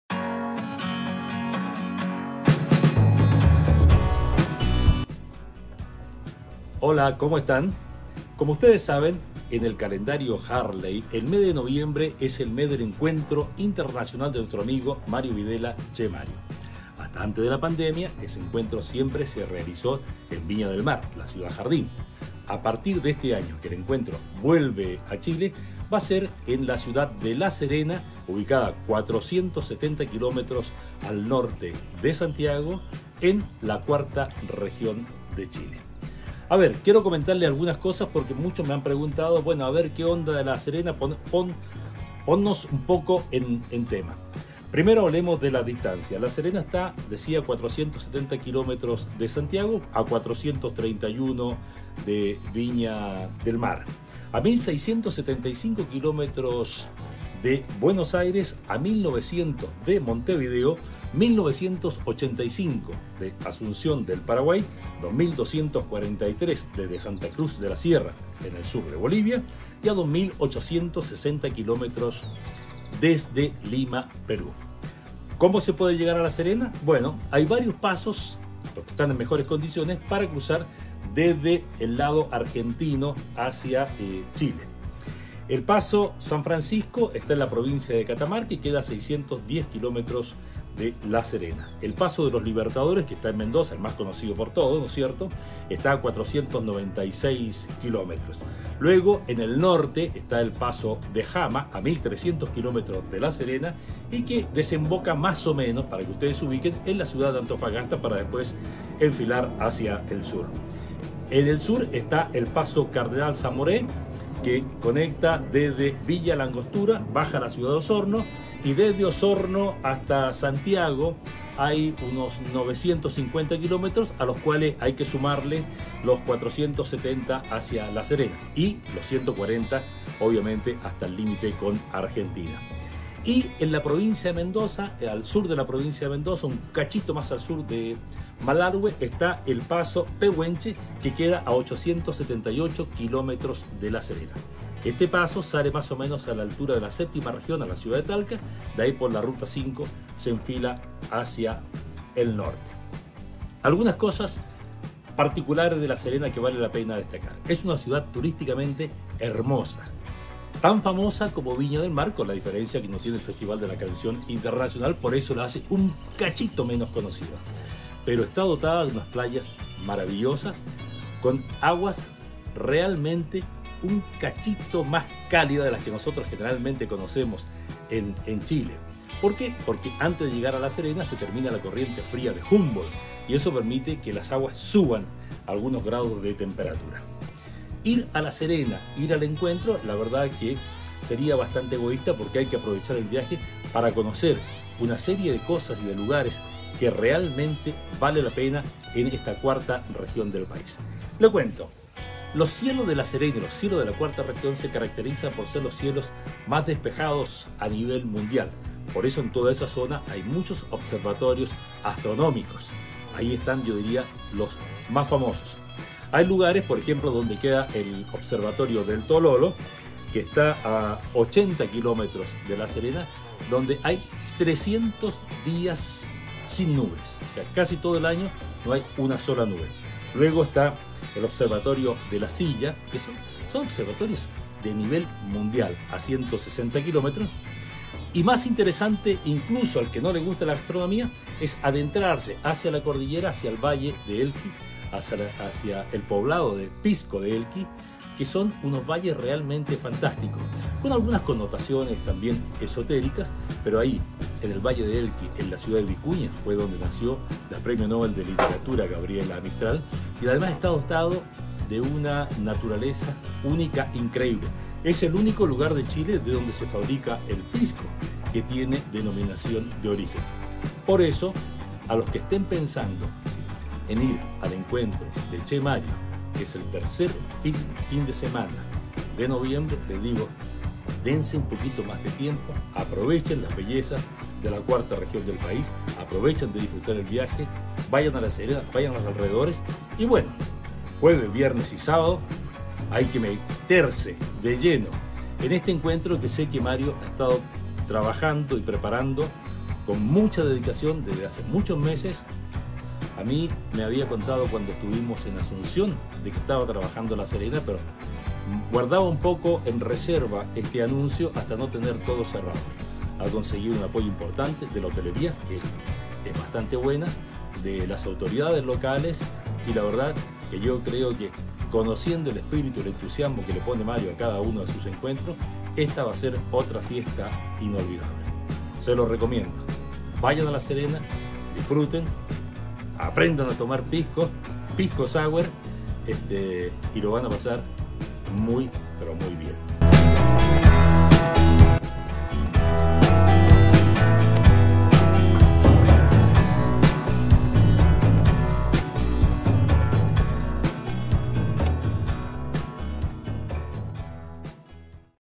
LA SERENA 2022, EL ENCUENTRO DE CHE MARIO
Este año vuelve a Chile el Encuentro Internacional Harley de Mario Videla, Che Mario.

La "cumbre" de harlista será el tercer fin de semana de Noviembre en la hermosa ciudad de La Serena, a 470 kilómetros al norte de Santiago.

Aquí te contamos porqué vale la pena ir a una de las ciudades balneario más hermosa de Chile.